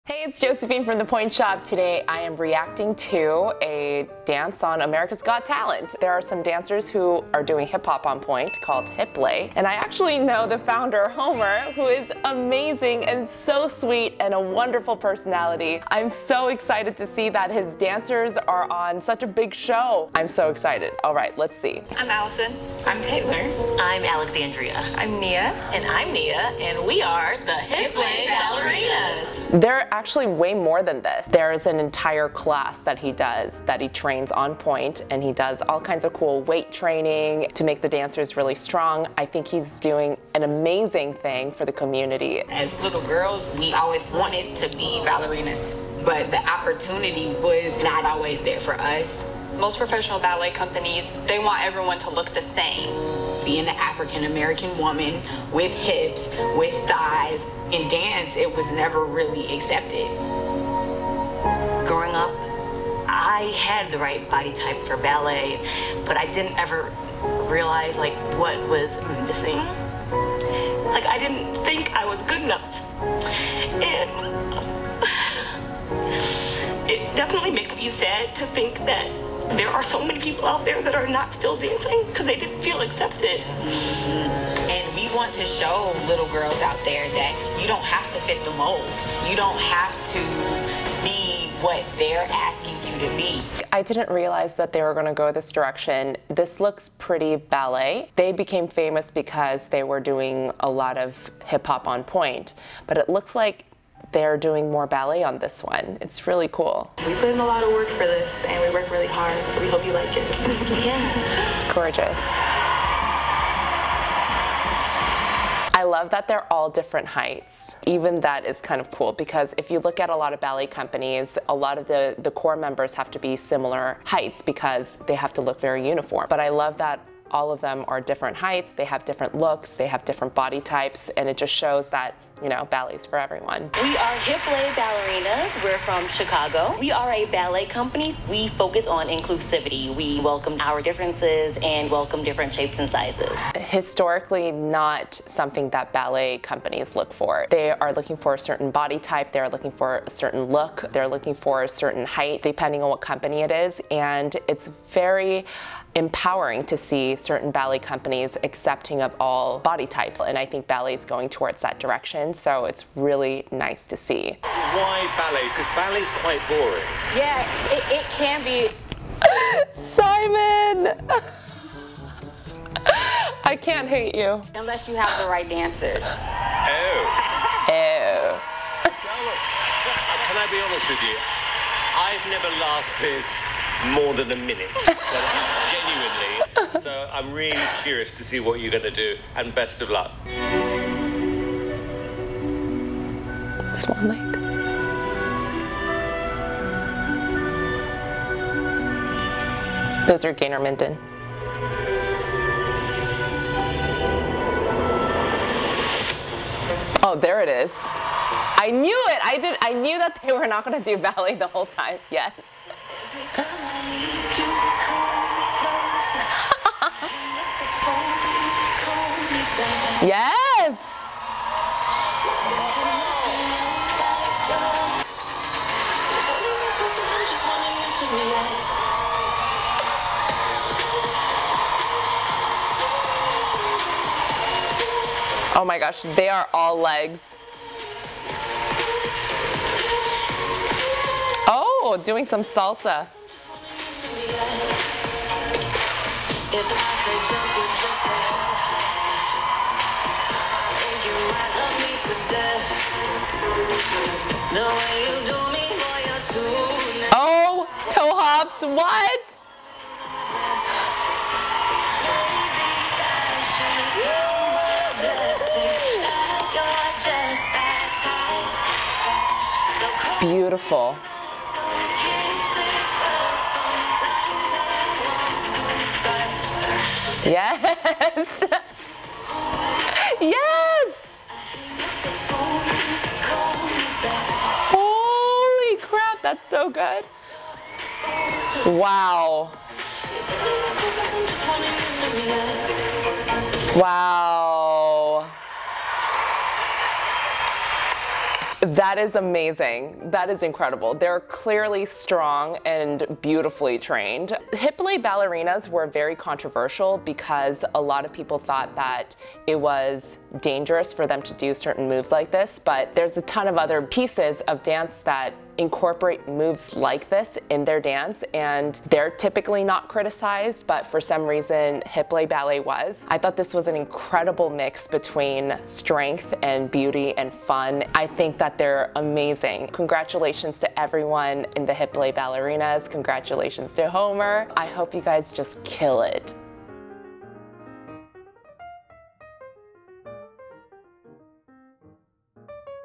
pointe shoe fitter reacts to HIPLET
"ballet is for everyone."

Want more  @JosephineLee ? Follow @josephineylee !